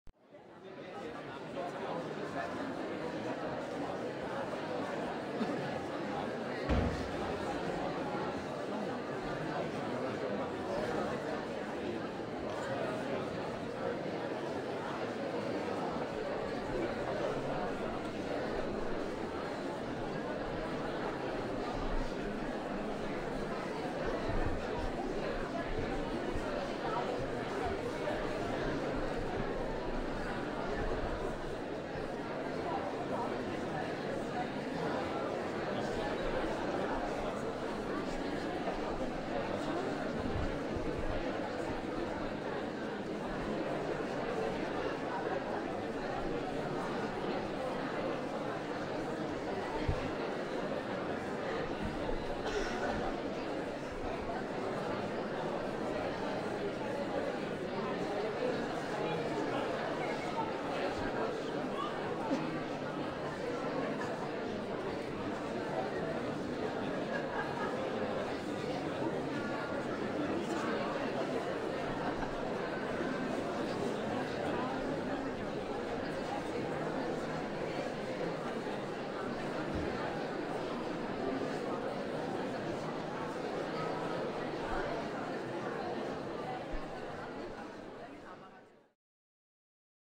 Crowd Talking Background Sound (HD) | Ambience for Videos and Projects
Engage your audience in the bustling atmosphere of a lively crowd with a captivating background sound. This handy ambient audio is the perfect addition to enhance your videos, films, podcasts, presentations, and various creative projects.

Whether you're recreating a bustling marketplace, a bustling cafe, or a city street scene, this crowd talking background sound will add depth and authenticity to your content.

Let your scenes come to life and create a more immersive experience for your viewers. The natural ebb and flow of voices will transport your audience to the heart of the action, making your content more engaging and relatable.